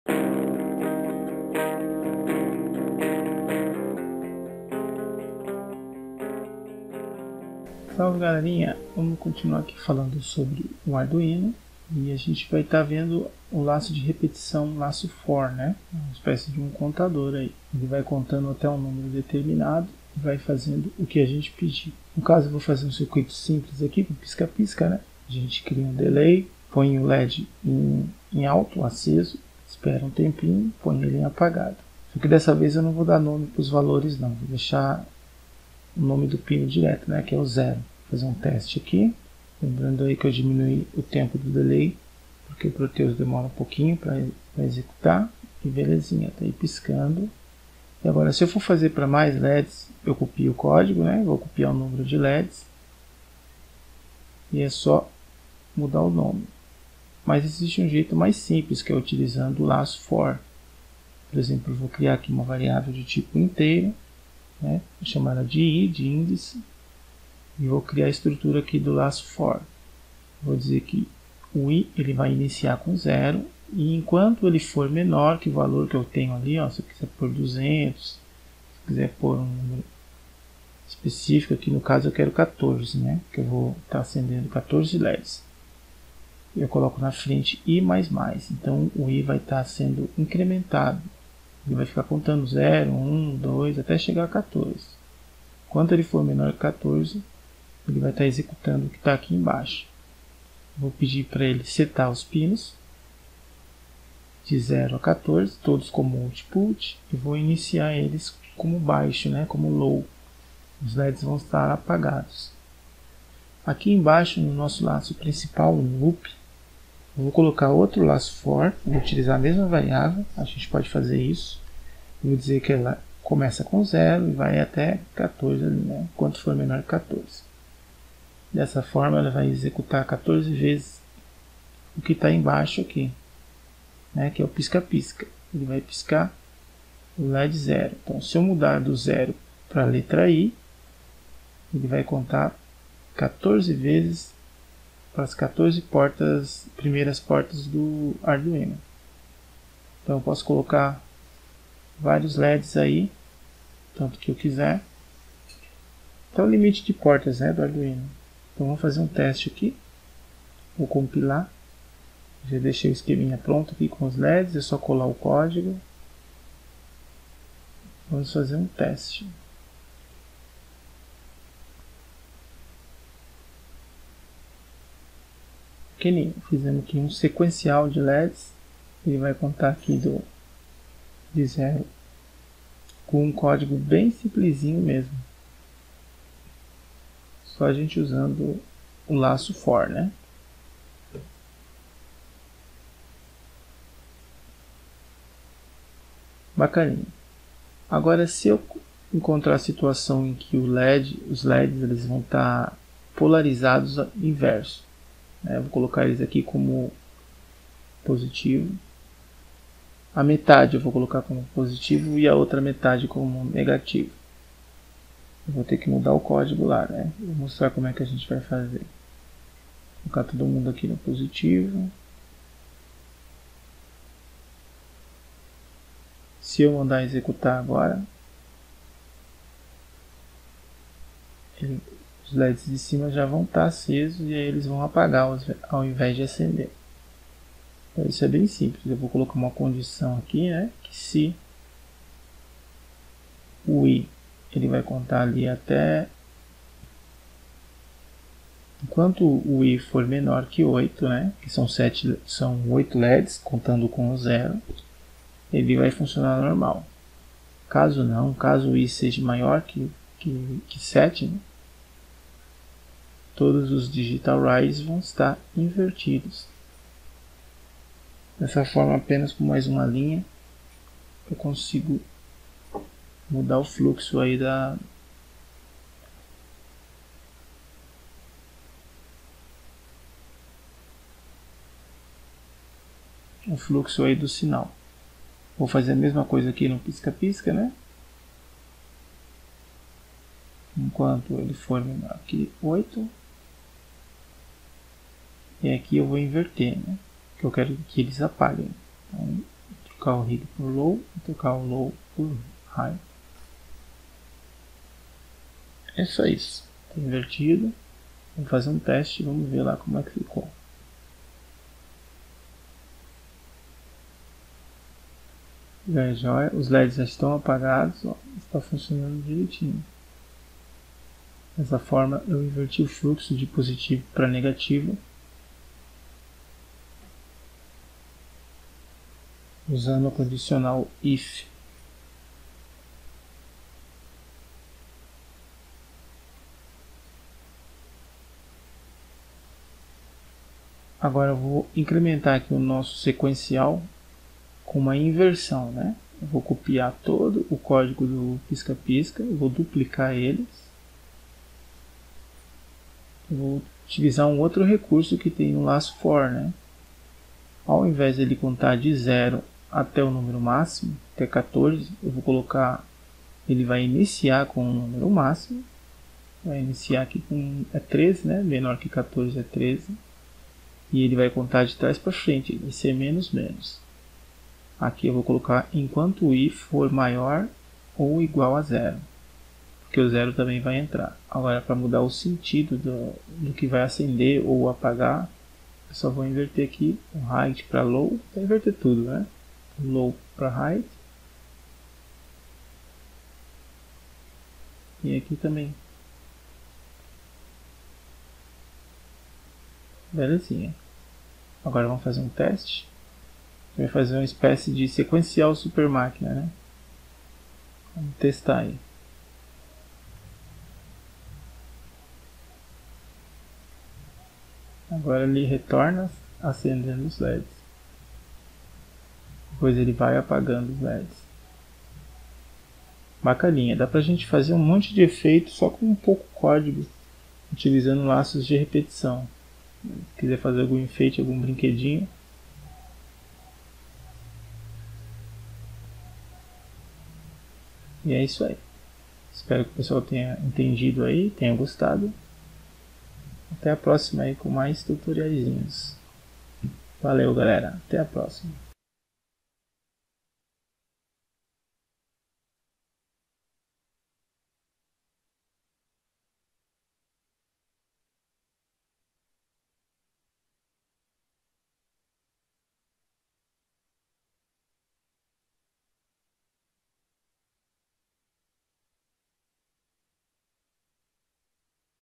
Arduino - 06 (Laço For)
Vamos aprender a controlar os pinos do Arduino do a estrutura de repetição for, criar variáveis de tipo inteiro e inverter o fluxo da corrente.

Canal destinado a programação e jogos em geral, ensinar e compartilhar conhecimento sobre programação computador em geral...